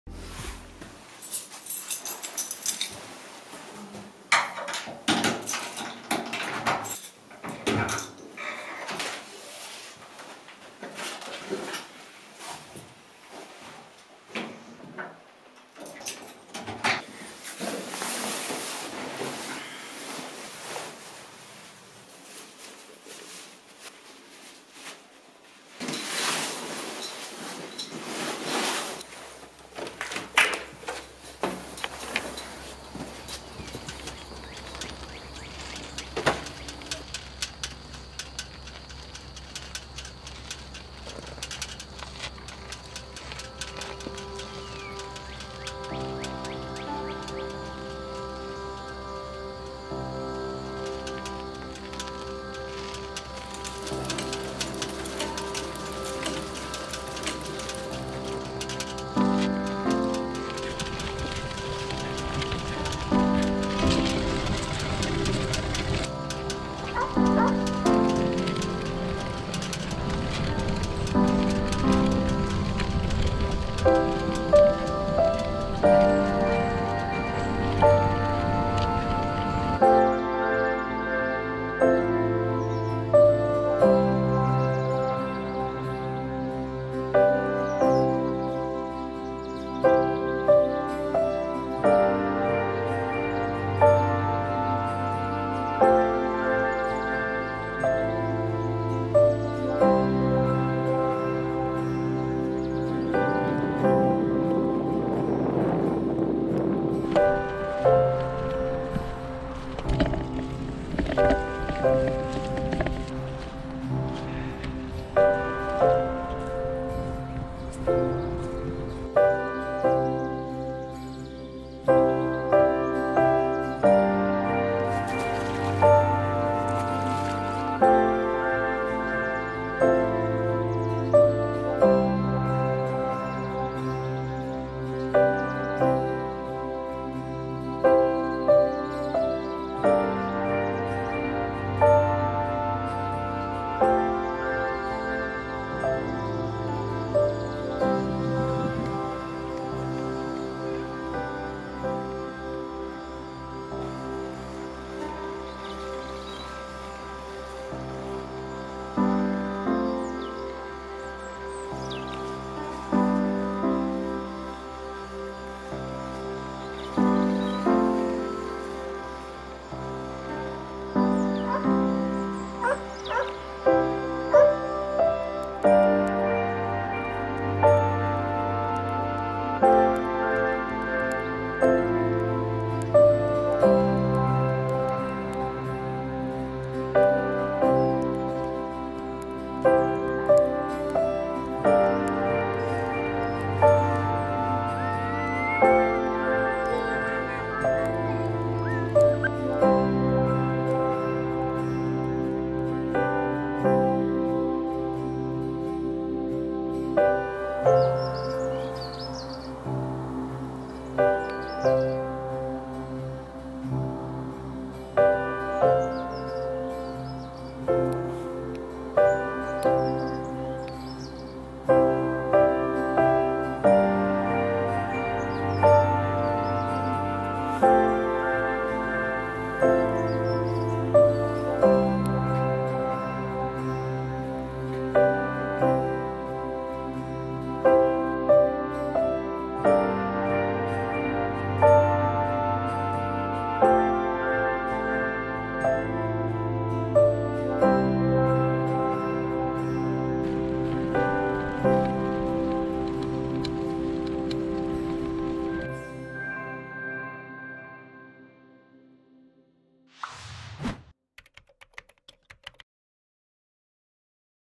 O AR DA MANHÃ parte 1 - Inglaterra 2019 │Lumix GH5 - cinematic 4K Ep.10
Nesse vídeo, você vai passear pelo interior da Inglaterra em busca de um amanhecer inspirador!! Relaxe e curta a natureza e os sons da manhã!!